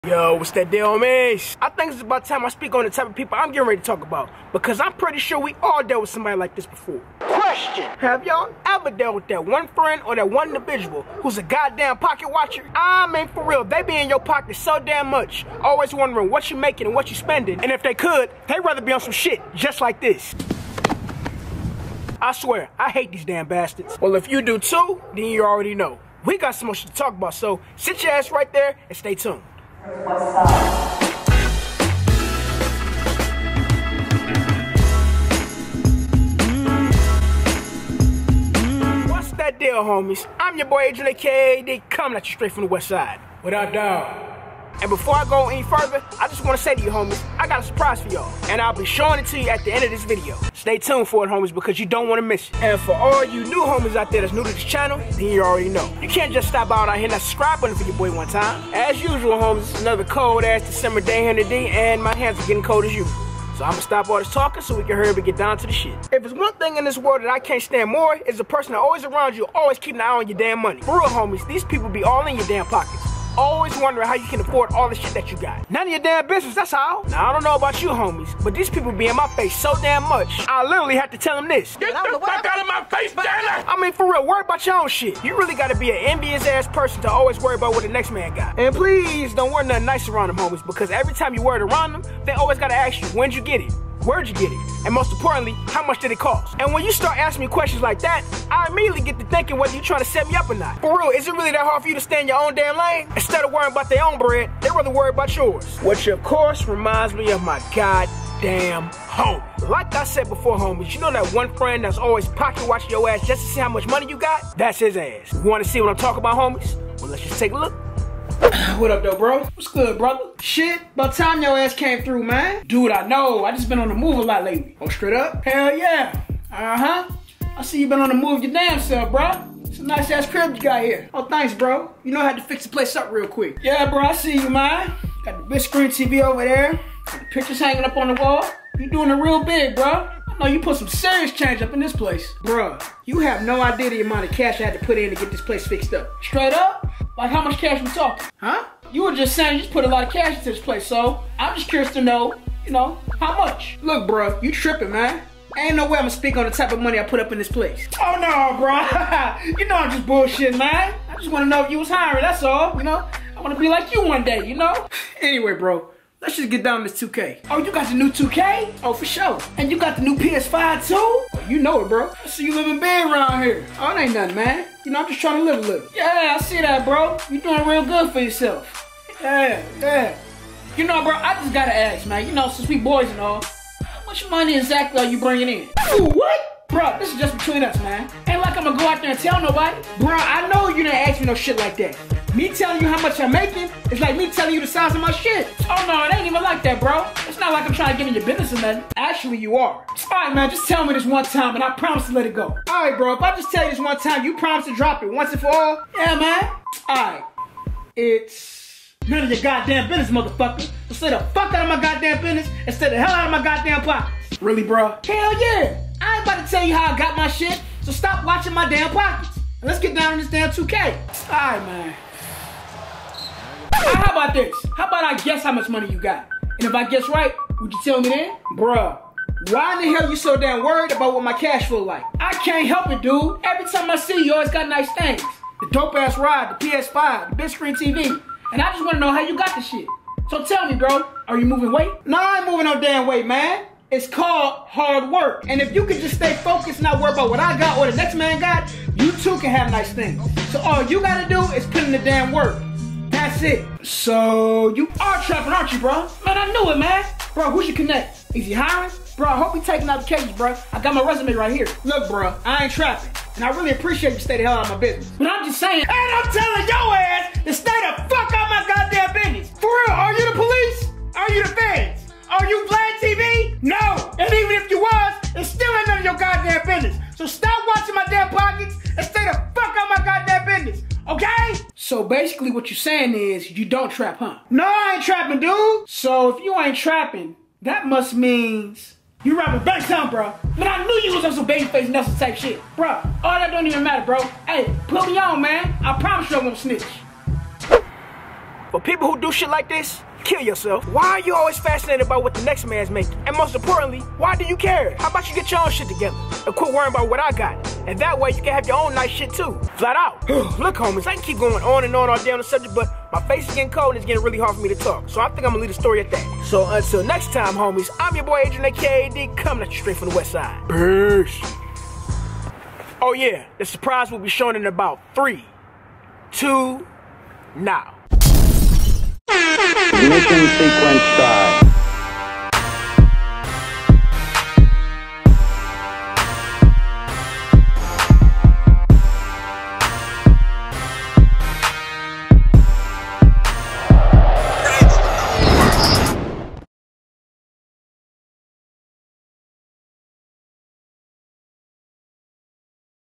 POCKET WATCHERS
Have you ever had that one friend that that's always watching in your pockets always wondering what you spend your money on?